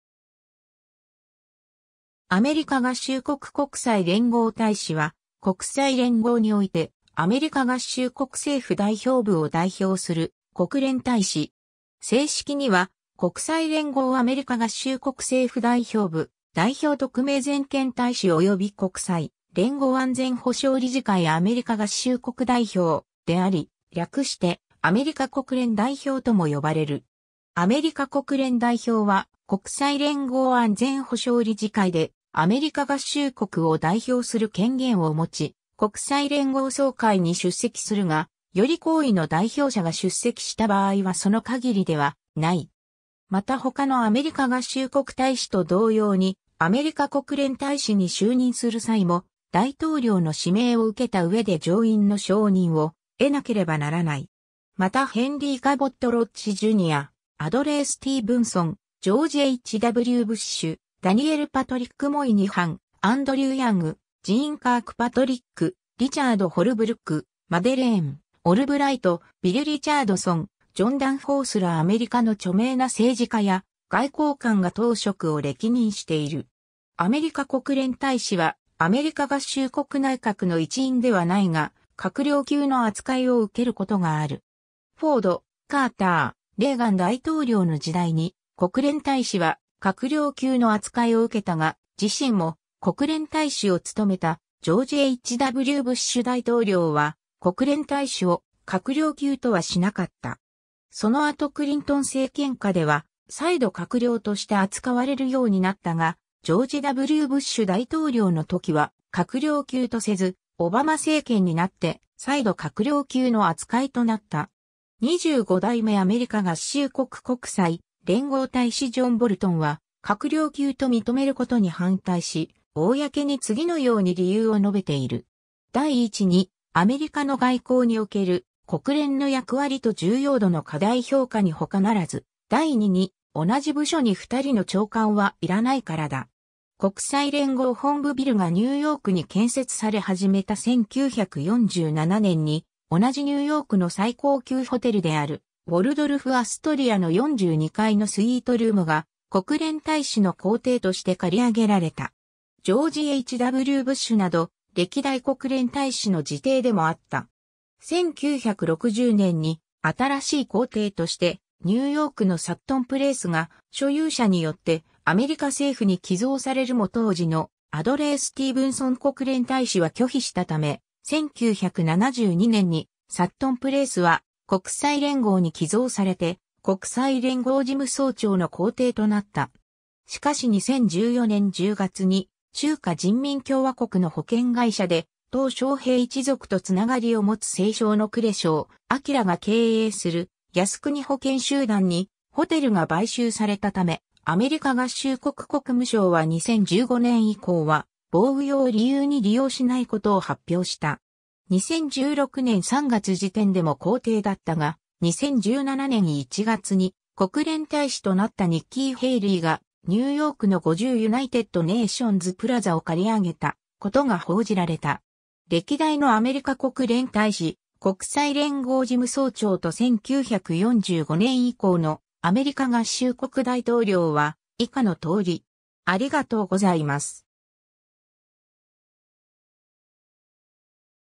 アメリカ合衆国国際連合大使
アメリカ合衆国・国際連合関係
アメリカ合衆国国際連合大使（アメリカがっしゅうこくこくさいれんごうたいし、英語: United States Ambassador to the United Nations）は、国際連合においてアメリカ合衆国政府代表部を代表する国連大使。
 正式には「国際連合アメリカ合衆国政府代表部代表特命全権大使及び国際連合安全保障理事会アメリカ合衆国代表」であり、略してアメリカ国連代表とも呼ばれる。
 アメリカ国連代表は国際連合安全保障理事会でアメリカ合衆国を代表する権限を持ち、国際連合総会に出席するが、より高位の代表者（アメリカ合衆国国務長官やアメリカ合衆国大統領）が出席した場合はその限りではない。
また他のアメリカ合衆国大使と同様にアメリカ国連大使に就任する際も、大統領の指名を受けた上で上院の承認を得なければならない。
 またヘンリー・カボット・ロッジ・ジュニア、アドレー・スティーブンソン、ジョージ・H・W・ブッシュ、ダニエル・パトリック・モイニハン、アンドリュー・ヤング、ジーン・カークパトリック、リチャード・ホルブルック、マデレーン・オルブライト、ビル・リチャードソン、ジョン・ダンフォースらアメリカの著名な政治家や外交官が当職を歴任している。
 アメリカ国連大使はアメリカ合衆国内閣の一員ではないが、閣僚級の扱いを受けることがある。
フォード、カーター、レーガン大統領の時代に国連大使は閣僚級の扱いを受けたが、自身も国連大使を務めたジョージ・H・W・ブッシュ大統領は国連大使を閣僚級とはしなかった。
その後クリントン政権下では再度閣僚として扱われるようになったが、ジョージ・W・ブッシュ大統領の時（2001年1月20日から2009年1月20日まで）は閣僚級とせず、オバマ政権になって再度閣僚級の扱いとなった。
25代目アメリカ合衆国国際連合大使ジョン・ボルトンは閣僚級と認めることに反対し、公に次の様に理由を述べている。
「第一に、（国連大使を閣僚級とすることは）アメリカの外交における国連の役割と重要度の過大評価に他ならず、第二に、同じ部署（国務省）に2人の長官はいらないからだ。」 国際連合本部ビルがニューヨークに建設され始めた1947年に同じニューヨークの最高級ホテルであるウォルドルフ＝アストリアの42階のスイートルームが国連大使の公邸として借り上げられた。
ジョージ・H・W・ブッシュなど歴代国連大使の自邸でもあった。
 1960年に新しい公邸としてニューヨークのサットン・プレイスが所有者によってアメリカ政府に寄贈されるも当時のアドレー・スティーブンソン国連大使は拒否したため、1972年にサットン・プレイスは国際連合に寄贈されて国際連合事務総長の公邸となった。
 しかし2014年10月に中華人民共和国の保険会社で鄧小平一族と繋がりを持つ政商の呉小暉が経営する安邦保険集団にホテルが買収されたため、アメリカ合衆国国務省は2015年以降は防諜を理由に利用しないことを発表した。
2016年3月時点でも公邸だったが、2017年1月に国連大使となったニッキー・ヘイリーがニューヨークの50 United Nations Plazaを借り上げたことが報じられた。
 歴代のアメリカ国連大使（代行を含む）・国際連合事務総長と1945年以降のアメリカ合衆国大統領は以下の通り。